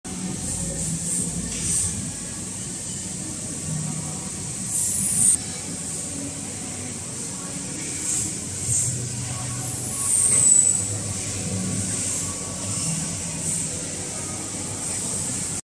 Inflatable 360 photo booth cabin
Inflatable 360 cabin, 10ft*10ft (3m*3m).
Material: Oxford cloth
Configuration: fan, sandbag, light bar (16 colors), adapter, remote control, controller.